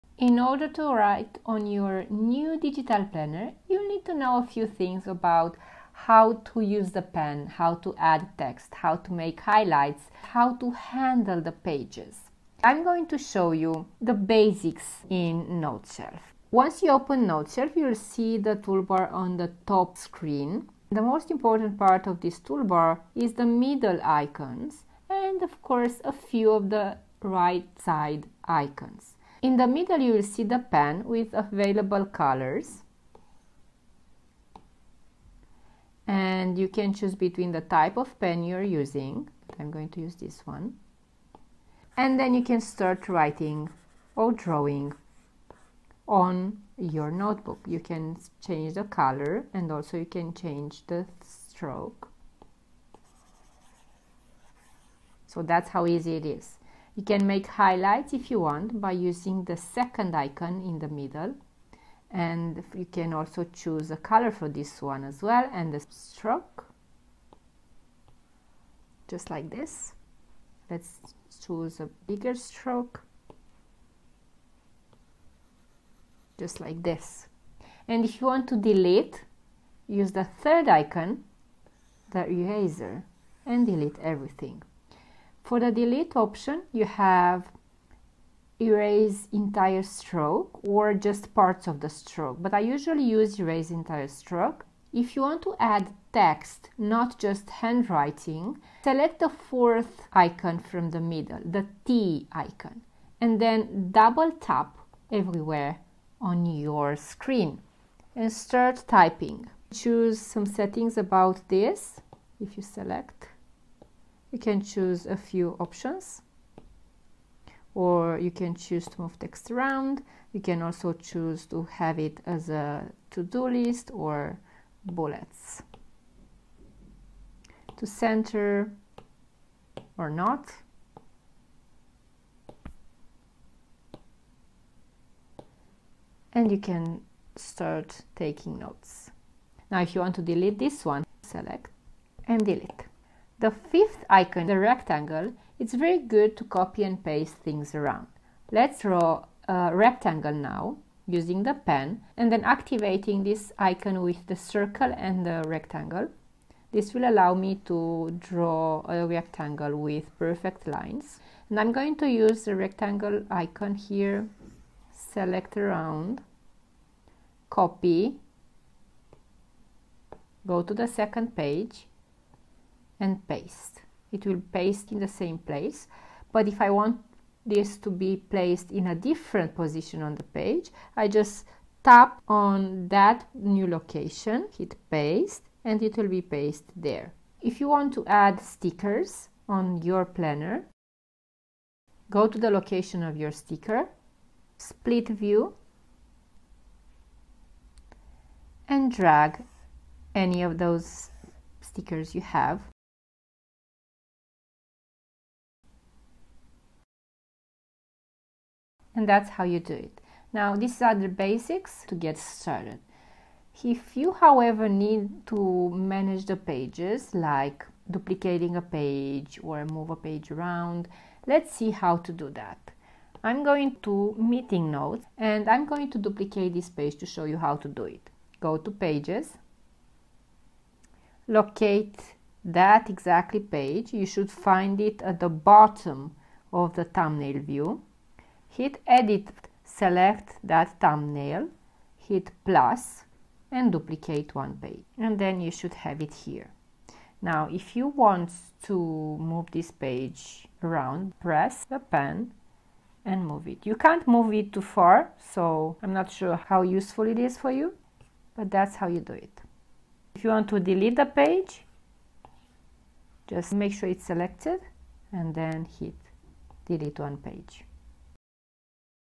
Be a digital note-taking hero → check the main steps I use to take digital notes in Noteshelf
Keep up with the note-taking trends. It's time to get comfortable with digital note-taking, so check the main steps I use to take digital notes in Noteshelf.